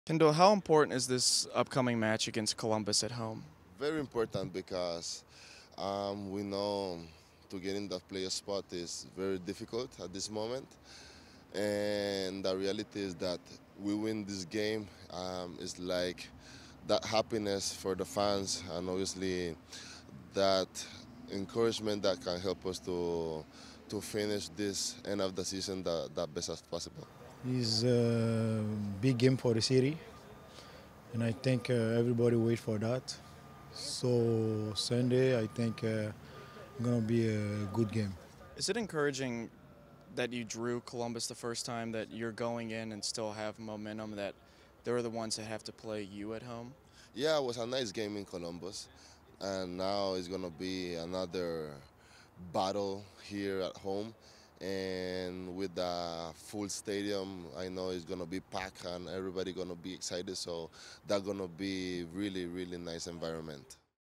Waston and Lamah on Columbus Crew SC
Kendall Waston and Roland Lamah speak ahead of taking on Columbus Crew SC at Nippert Stadium.